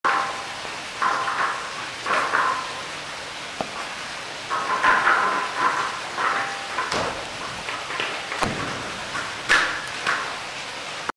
Cleo kicks and hits a buoy.
Cleo kicks and hits a buoy. Recorded on January 21, 2007 in Kyoto University Primate Research Institute/Global COE